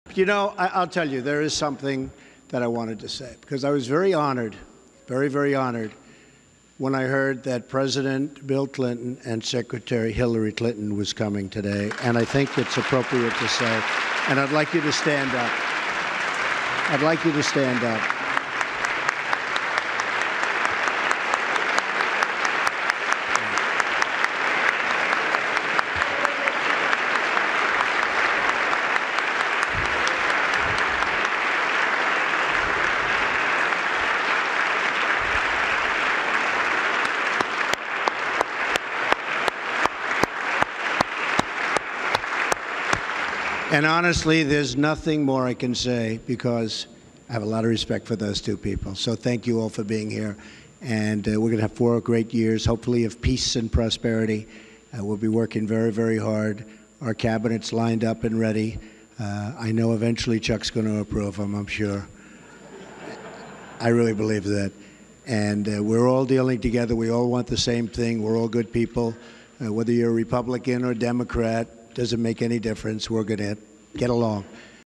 Trump honors Clintons during luncheon remarks
During his remarks at the Inauguration Day luncheon, President Donald Trump said he was honored that former President Bill Clinton and Hillary Clinton were in attendance.